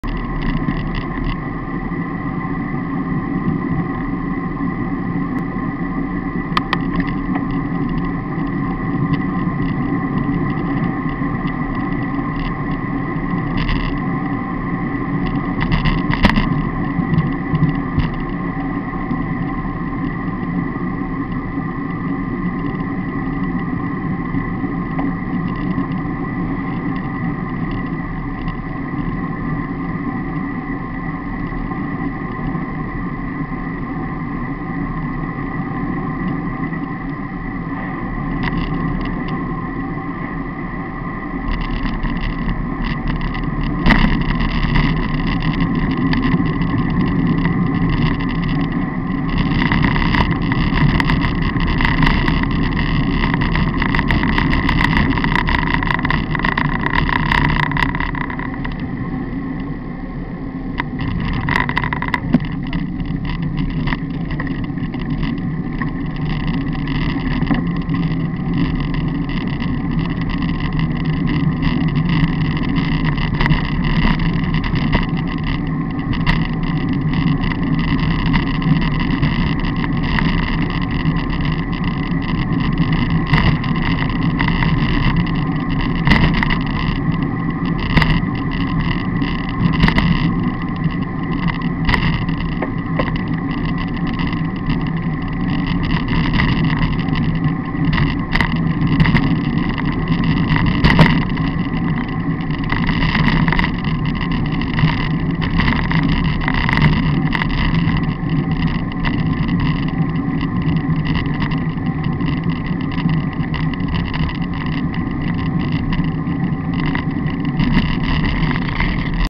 Limburg 1 001
Een dag gefietst in het mooie limburg. 1